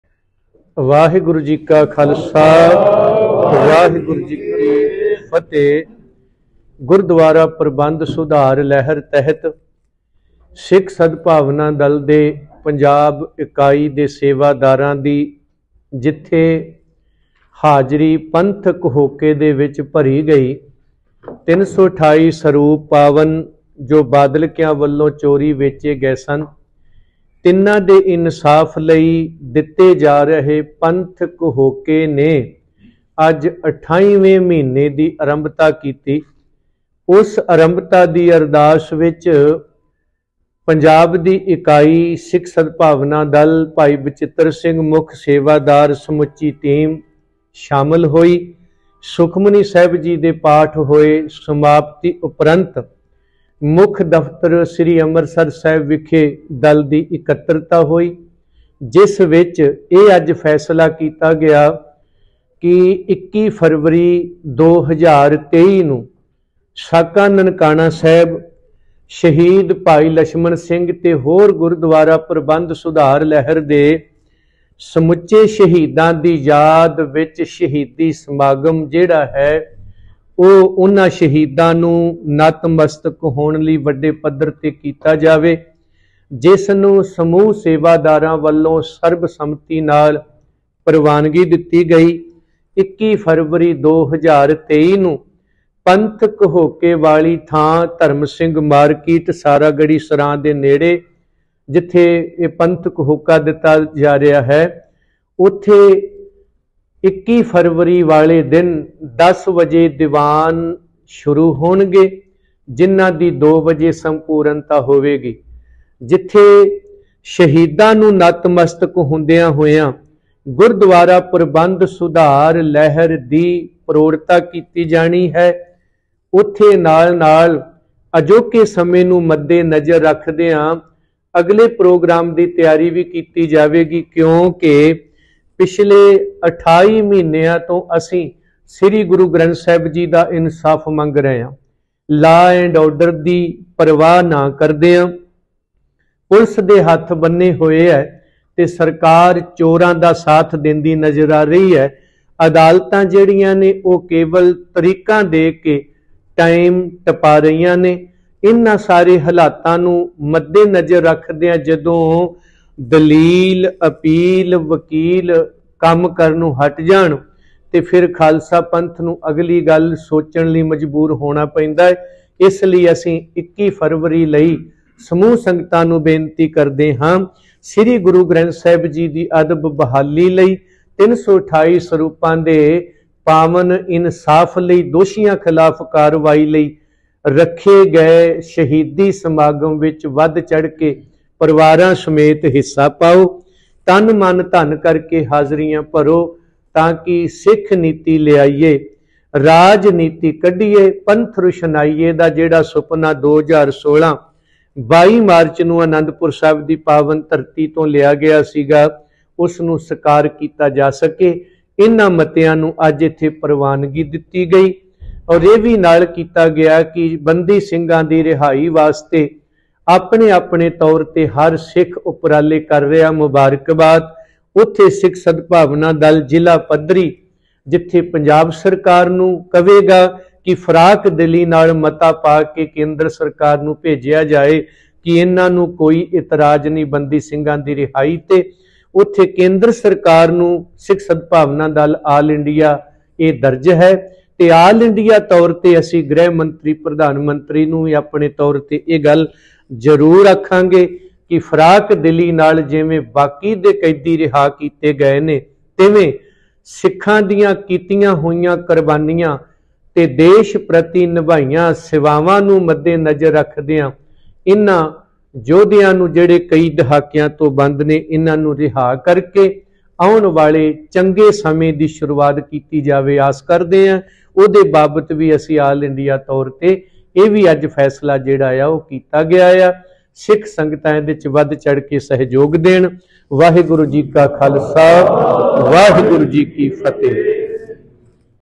ਸਾਕਾ ਨਨਕਾਣਾ ਸਾਹਿਬ ਨੂੰ ਸਮਰਪਿਤ, ੨੧ ਫਰਵਰੀ ਨੂੰ ਹੋਵੇਗਾ ਸ਼ਹੀਦੀ ਸਮਾਗਮ, ਭਾਈ ਵਡਾਲਾ
ਸਾਕਾ ਨਨਕਾਣਾ ਸਾਹਿਬ ਨੂੰ ਸਮਰਪਿਤ
੨੧ ਫਰਵਰੀ ਨੂੰ ਹੋਵੇਗਾ ਸ਼ਹੀਦੀ ਸਮਾਗਮ- ਭਾਈ ਵਡਾਲਾ

੨੧ ਫਰਵਰੀ ਨੂੰ ਵੱਡੇ ਪੱਧਰ ਤੇ ਸਿੱਖ ਸੰਗਤਾਂ ਕਰਨ ਸਮੂਲੀਅਤ -ਭਾਈ ਬਚਿੱਤਰ ਸਿੰਘ ਸੰਗਰੂਰ- ਪੰਜਾਬ ਪ੍ਰਧਾਨ ।

ਸ੍ਰੀ ਅੰਮ੍ਰਿਤਸਰ ਸਾਹਿਬ
ਅੱਜ ਮਿਤੀ ੪ ਫਰਵਰੀ ਨੂੰ ਪੰਥਕ ਹੋਕੇ ਦੇ ਅਠਾਈਵੇਂ ਮਹੀਨੇ ਦੀ ਅਰੰਭਤਾ ਕਰਦਿਆਂ ੩੨੮ ਪਾਵਨ ਸਰੂਪਾਂ ਦੇ ਇਨਸਾਫ ਲਈ ਦੋਸ਼ੀਆ ਖਿਲਾਫ ਕਾਰਵਾਈ ਲਈ ਨਕੋਦਰ ਕਾਂਡ ਦੇ ਸ਼ਹੀਦਾਂ ਨੂੰ ਸਮਰਪਿਤ ਪੰਥਕ ਹੋਕੇ ਦੇ ਦੀਵਾਨ ਸਜਾਏ ਗਏ। ਸ੍ਰੀ ਸੁਖਮਨੀ ਸਾਹਿਬ ਚੌਪਈ ਸਾਹਿਬ ਜੀ ਦੇ ਪਾਠ ਹੋਏ।ਪੰਥਕ ਹੋਕੇ ਦੀ ਸਫਲਤਾ ਲਈ, ਗੁਰੂਧਾਮਾਂ ਦੀ ਬਹਾਲੀ ਲਈ, ਬੰਦੀ ਸਿੰਘ ਰਿਹਾਈ ਲਈ ਅਰਦਾਸ ਹੋਈ। ਜਿਸ ਵਿੱਚ ਦਲ ਦੀ ਪੰਜਾਬ ਪੱਧਰੀ ਇਕਾਈ ਨੇ ਹਾਜਰੀ ਭਰੀ।
ਉਪਰੰਤ ਮੁੱਖ ਦਫਤਰ ਸੁਲਤਾਨਵਿੰਡ ਵਿਖੇ। ਭਾਈ ਬਲਦੇਵ ਸਿੰਘ ਵਡਾਲਾ ਕੌਮੀ ਸੇਵਾਦਾਰ ਸਿੱਖ ਸਦਭਾਵਨਾ ਦਲ ਹਜੂਰੀ ਕੀਰਤਨੀਏ ਸੱਚਖੰਡ ਸ੍ਰੀਦਰਬਾਰ ਸਾਹਿਬ ਅਤੇ ਭਾਈ ਬਚਿੱਤਰ ਸਿੰਘ ਸੰਗਰੂਰ ਮੁੱਖ ਸੇਵਾਦਾਰ ਪੰਜਾਬ ਦੀ ਅਗਵਾਈ ਹੇਠ ਇਕੱਤ੍ਰਤਾ ਕੀਤੀ ਗਈ। ਜਿਸ ਦੀ ਜਾਣਕਾਰੀ ਸਾਂਝੀ ਕਰਦਿਆਂ ਭਾਈ ਵਡਾਲਾ ਨੇ ਕਿਹਾ ਕਿ ਸਾਕਾ ਨਨਕਾਣਾ ਸਾਹਿਬ ਜੀ ਨੂੰ ਸਮਰਪਿਤ ਸ਼ਹੀਦ ਭਾਈ ਲਛਮਣ ਸਿੰਘ ਸਮੂਹ ਸ਼ਹੀਦਾਂ ਨੂੰ ਯਾਦ ਕਰਦਿਆਂ ਦਲ ਵੱਲੋ ੨੦੧੬-੨੨ ਮਾਰਚ ਨੂੰ ਸ੍ਰੀ ਅਨੰਦਪੁਰ ਸਾਹਿਬ ਤੋਂ ਗੁਰਦੁਆਰਾ ਪ੍ਰਬੰਧ ਸੁਧਾਰ ਲਹਿਰ ਦੀ ਅਰੰਭਤਾ ਕੀਤੀ ਗਈ ਸੀ,ਜਿਸ ਦੇ ਤਹਿਤ ੨੧ ਫਰਵਰੀ ੨੦੨੩ ,ਦਿੰਨ ਮੰਗਲਵਾਰ ੯ ਫੱਗਣ ਨੂੰ ਪੰਥਕ ਹੋਕੇ ਵਾਲੀ ਥਾਂ ਸ਼ਹੀਦੀ ਸਮਾਗਮ ਕੀਤਾ ਜਾਵੇਗਾ।ਜੋ ੩੨੮ ਪਾਵਨ ਸਰੂਪ,ਬੰਦੀ ਸਿੰਘ ਰਿਹਾਈ, ਬੇਅਦਬੀ ਕਾਂਡ, ਸ੍ਰੋਮਣੀ ਕਮੇਟੀ ਦੀ ਅਜਾਦੀ ਬਾਬਤ ਹੋਵੇਗਾ। ਕਿਹਾ ਜਿੱਥੇ ਬੰਦੀ ਸਿੰਘ ਰਿਹਾਈ ਲਈ ਸਿੱਖ ਆਪਣੇ-ਆਪਣੇ ਤੌਰ ਤੇ ਯਤਨ ਕਰ ਰਹੇ ਹਨ। ਉੱਥੇ ਦਲ ਵੱਲੋ ਵੀ ਹਰ ਤਰਾਂ ਦੇ ਯਤਨ ਜਾਰੀ ਰਹਿਣਗੇ। ਉਹਨਾ ਵੱਲੋ ਪੰਜਾਬ ਪੱਧਰੀ ਚੁਣੇ ਗਏ ਨੁਮਾਇੰਦਿਆ  ਨੂੰ ਨਿਯੁਕਤੀ ਪੱਤਰ ਵੀ ਸੌਂਪੇ,ਭਾਈ ਬਚਿੱਤਰ ਸਿੰਘ ਸੰਗਰੂਰ ਮੁੱਖ ਸੇਵਾਦਾਰ, ਭਾਈ ਗੁਰਮੀਤ ਸਿੰਘ ਥੂਹੀ ਮੀਤ ਸੇਵਾਦਾਰ, ਭਾਈ ਬਲਜੀਤ ਸਿੰਘ ਸ਼ਿਕਾਰ ਮਾਛੀਆਂ ਜਰਨਲ ਸਕੱਤਰ, ਭਾਈ ਅਮਰਪਾਲ ਸਿੰਘ ਫਰੀਦਕੋਟ ਖਜ਼ਾਨਚੀ, ਭਾਈ ਗੁਰਵਤਨ ਸਿੰਘ ਜੀ ਮੁਕੇਰੀਆ ਸਹਾਇਕ ਖਜ਼ਾਨਚੀ, ਭਾਈ ਅਵਤਾਰ ਸਿੰਘ ਜੀ ਖਾਲਸਾ ਬਕਾਪੁਰ, ਭਾਈ ਅਰਵਿੰਦਰ ਸਿੰਘ ਜੀ ਤਰਨਤਾਰਨ, ਭਾਈ ਰਾਜਨ ਸਿੰਘ ਜੀ ਬਠਿੰਡਾ,ਭਾਈ ਚਰਨਜੀਤ ਸਿੰਘ ਜੀ ਜਲੰਧਰ, ਭਾਈ ਗੁਰਵਿੰਦਰ ਸਿੰਘ ਜੀ ਮਲੇਰ ਕੋਟਲਾ, ਭਾਈ ਰਾਜਵੰਤ ਸਿੰਘ ਜੀ ਨਵਾਂਸ਼ਹਿਰ, ਅੰਤ੍ਰਿਗ ਸਭਾ, ਭਾਈ ਜਰਨੈਲ ਸਿੰਘ ਜੀ ਖਰੜ, ਭਾਈ ਗੁਰਜੀਤ ਸਿੰਘ ਜੀ ਬਰਨਾਲਾ, ਭਾਈ ਨਰਿੰਦਰਪਾਲ ਸਿੰਘ ਜੀ ਜਲੰਧਰ , ਭਾਈ ਇਕਬਾਲ ਸਿੰਘ ਜੀ ਫ਼ਿਰੋਜ਼ਪੁਰ , ਭਾਈ ਪਰਮਿੰਦਰ ਸਿੰਘ ਜੀ ਪਠਾਨਕੋਟ , ਭਾਈ ਬਲਰਾਜ ਸਿੰਘ ਜੀ  ਫਰੀਦਕੋਟ ਭਾਈ ਸੁਖਦੇਵ ਸਿੰਘ ਜੀ ਵੈਦ ,ਭਾਈ ਮਨਪ੍ਰੀਤ ਸਿੰਘ ਜੀ , ਭਾਈ ਇਕਬਾਲ ਸਿੰਘ ਜੀ ਮਨਾਵਾਂ ,  ਭਾਈ ਗਗਨਪ੍ਰੀਤ ਸਿੰਘ ਜੀ , ਮਾਤਾ ਕੰਵਲਜੀਤ ਕੌਰ ਜੀ ਵਿਸ਼ੇਸ਼ ਕਰਕੇ ਹਾਜਿਰ ਹੋਏ ।
ਸਦੀਵੀ ਹੱਲ ਲਈ ਇਹ ਵੀ ਪ੍ਰਵਾਨ ਹੋਇਆ ਸਿੱਖਨੀਤੀਵਾਨ ਲਿਆਵਾਂਗੇ, ਰਾਜਨੀਤਕ ਕੱਢਾਂਗੇ, ਪੰਥ ਰੁਸ਼ਨਾਵਾਂਗੇ,